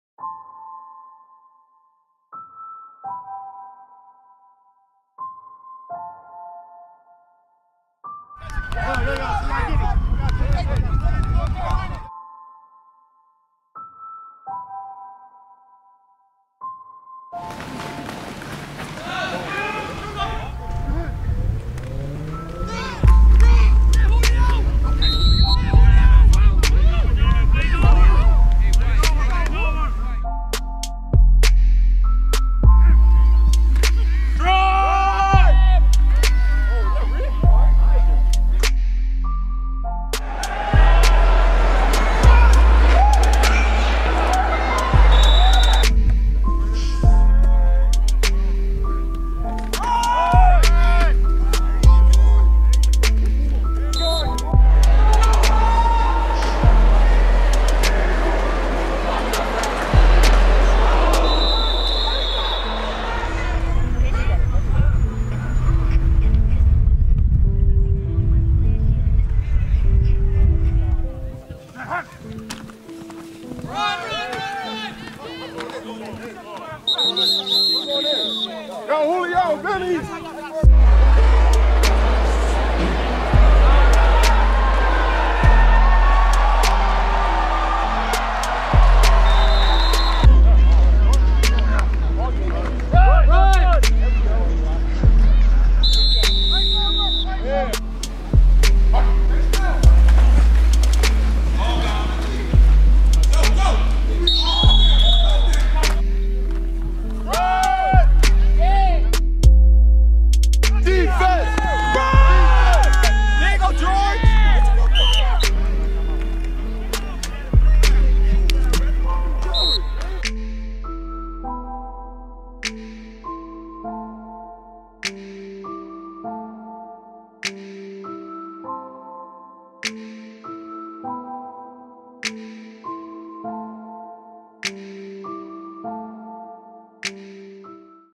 👼🏾 R.I.P. Julio Ferrer
Julio Ferrer was tragically shot and killed in York, PA on December 16th, 2017. He was 31 years old. May his soul rest in peace.

Here are some clips I captured from his 2017 season(s) with the York Silver Bullets, Central Penn Chargers, and York Generals

Filmed & Edited By: Brock Lane
Personal Instagram: @Brock_Lanez
Personal Facebook: Brock Lane